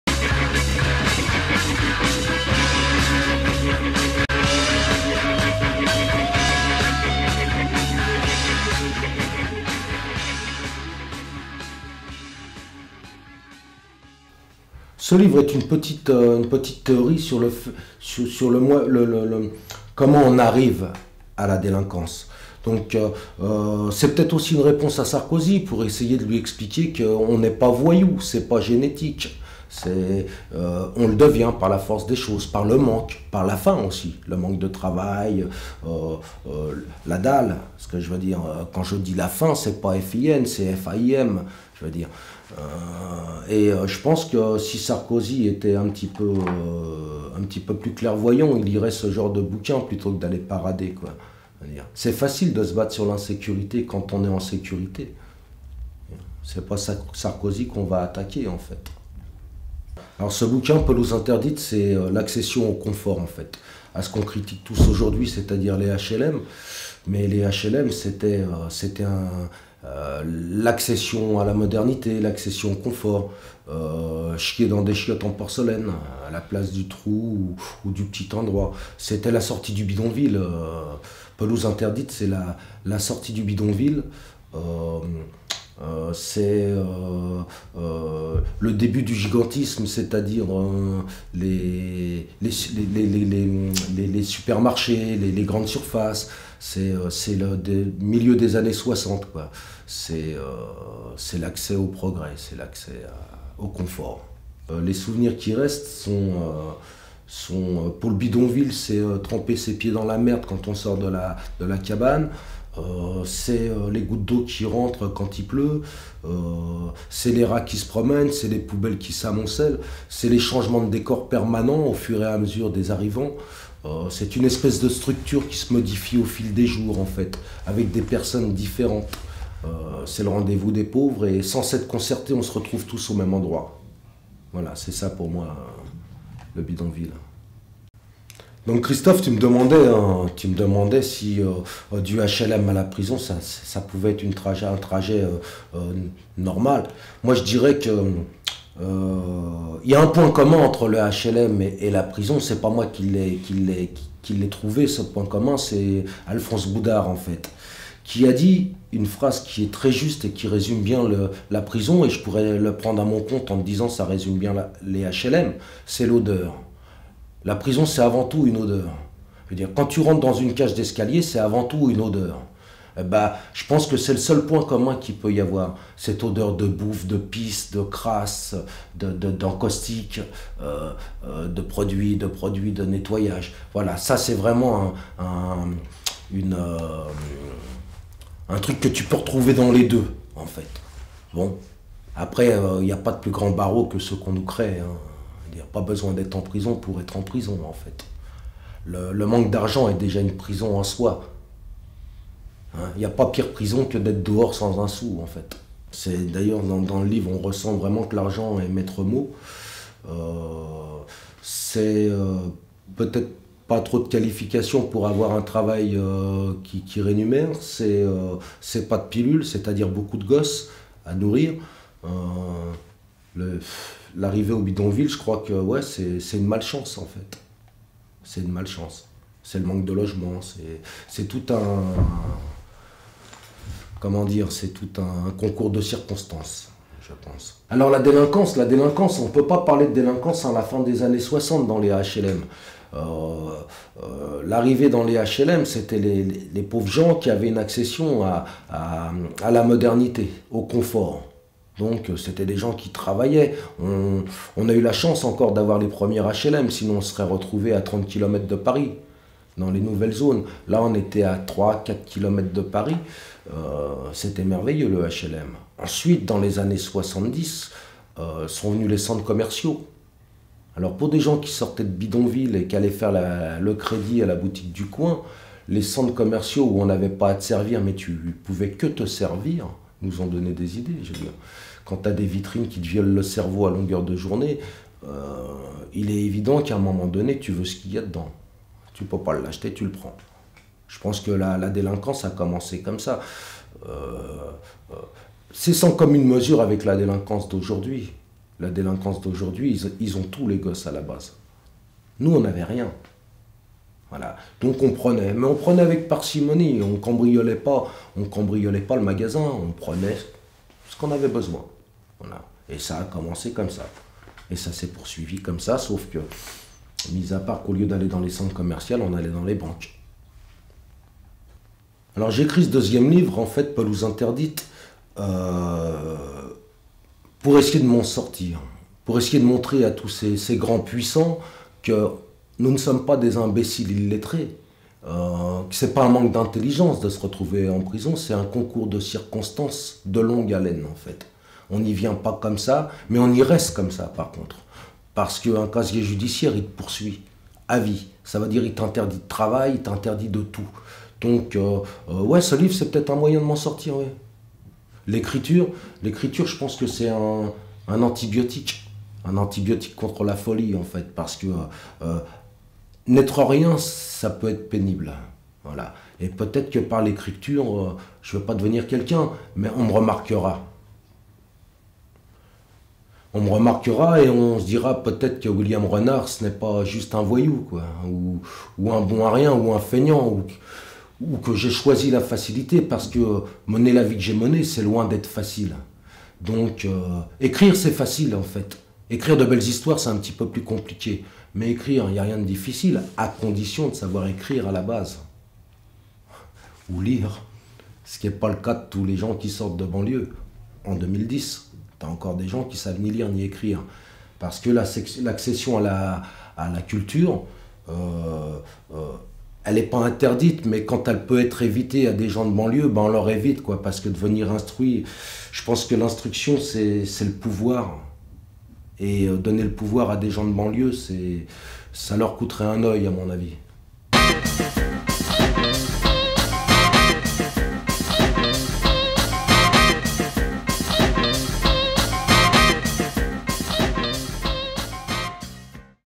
"Pelouse interdite" de William Renard
William Renard est devenu écrivain en prison.
Dans "Pelouse interdite"  il décrit le passage du bidonville aux premiers HLM.
Il parle d'une misère et des conditions qui font qu'un enfant devient délinquant pour ne plus voir sa mère pleurer et pour ne plus avoir faim. Pour lui Nicolas Sarkozy devrait lire son livre avant de réprimer les banlieues.